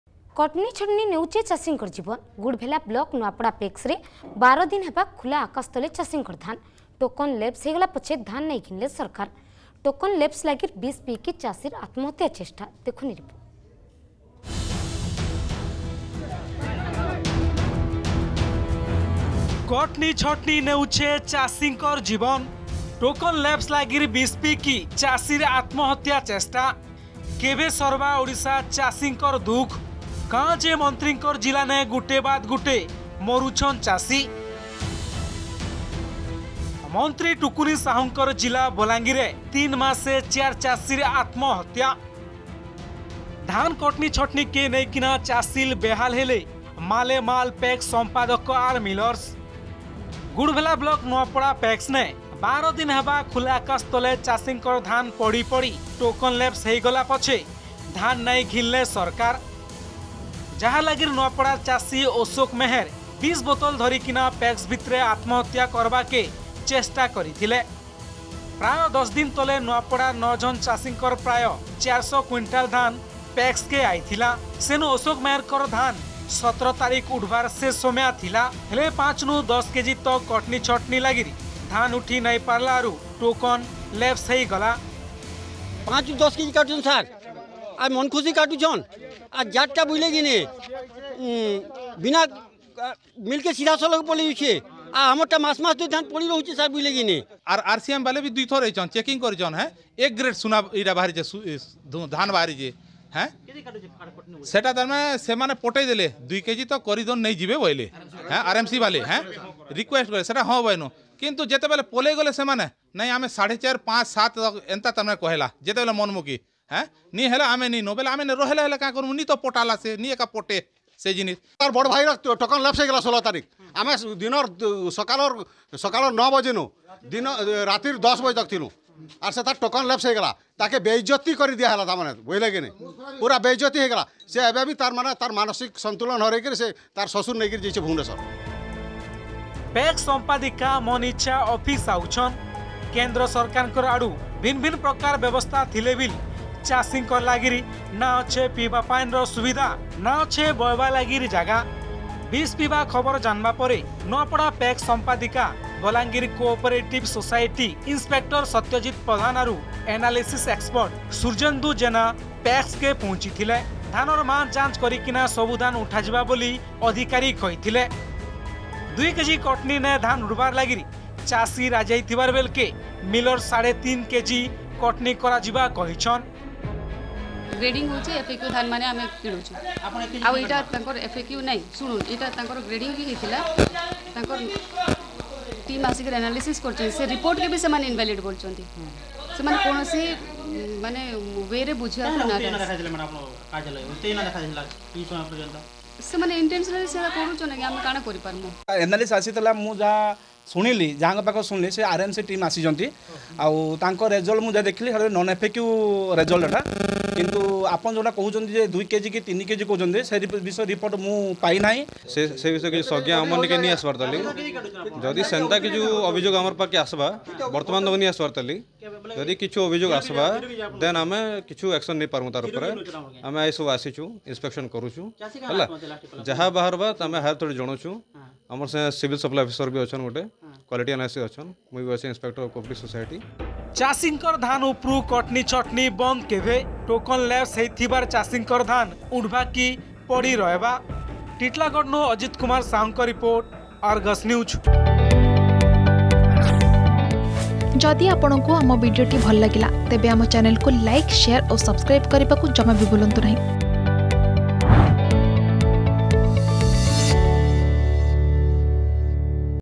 କଟନୀ ଛଟନୀ ନେଉଛେ ଚାଷୀକଁର୍ ଜୀବନ୍ || Farmers paddy Problem
କଟନୀ ଛଟନୀ ନେଉଛେ ଚାଷୀକଁର୍ ଜୀବନ୍ । ଗୁଡ଼ଭେଲା ବ୍ଳକ ନୂଆପଡ଼ା ପେକ୍ସରେ ୧୨ ଦିନ୍ ହେବା ଖୁଲା ଆକାଶ ତଲେ ଚାଷୀକଁର୍ ଧାନ୍ । ଟୋକନ୍ ଲେପ୍ସ ହେଇଗଲା ପଛେ ଧାନ୍ ନାଇଁ ଘିନଲେ ସରକାର୍ । ଟୋକନ୍ ଲେପ୍ସ ଲାଗିର୍ ବିଷ ପିଇକି ଚାଷୀର୍ ଆତ୍ମହତ୍ୟା ଚେଷ୍ଟା ।